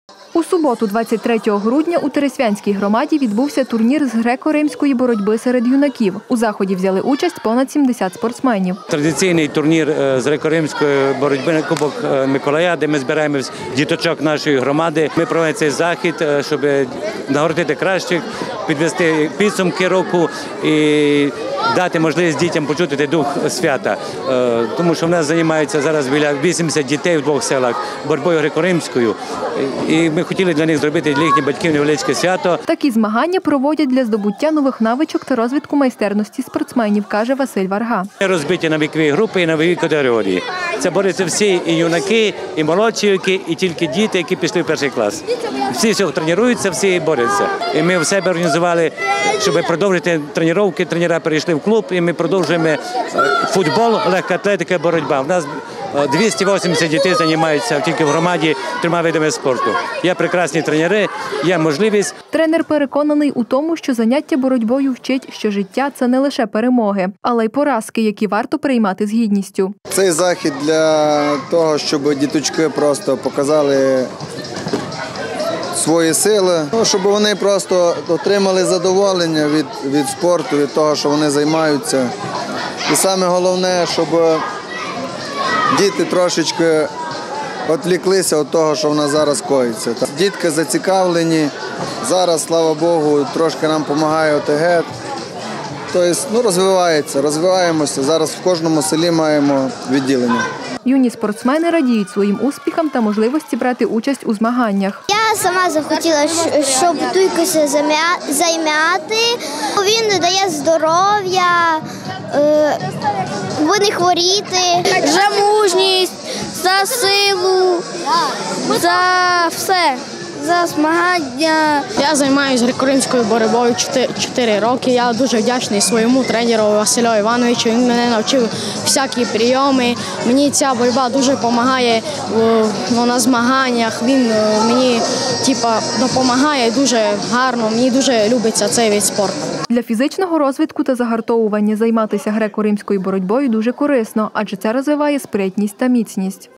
Турнір з греко – римської боротьби
У суботу, 23 грудня у Тересвянській громаді відбувся турнір з греко – римської боротьби серед юнаків. У заході взяли участь понад 70 спортсменів. Такі змагання проводять для здобуття нових навичок та розвитку майстерності спортсменів, каже Василь Варга. Тренер переконаний у тому, що заняття боротьбою вчить, що життя – це не лише перемоги, але й поразки, які варто приймати з гідністю.Юні спортсмени радіють своїм успіхам та можливості брати участь у змаганнях. Для фізичного розвитку та загартування займатися греко – римською боротьбою дуже корисно, адже це розвиває спритність та міцність.